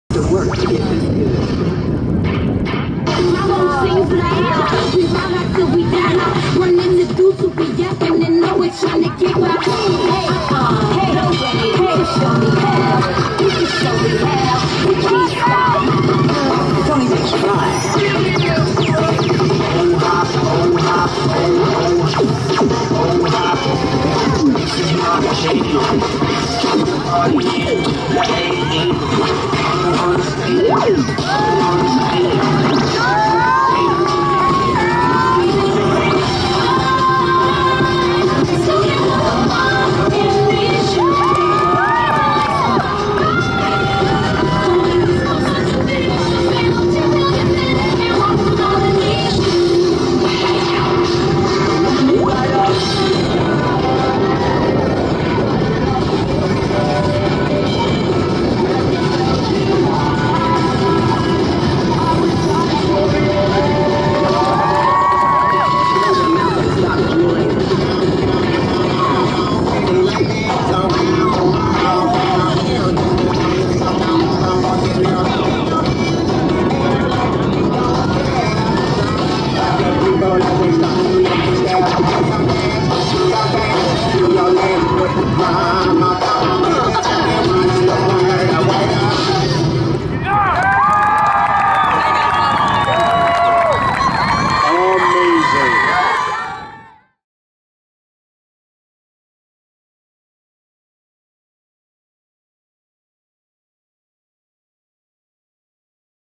141003 GRANADA HIGH JV CHEERLEADERS (HOMECOMING)
The performance of the Granada Matador JV Cheerleaders at half-time of the JV game on Homecoming, October 3, 2014.  More photos and video of school events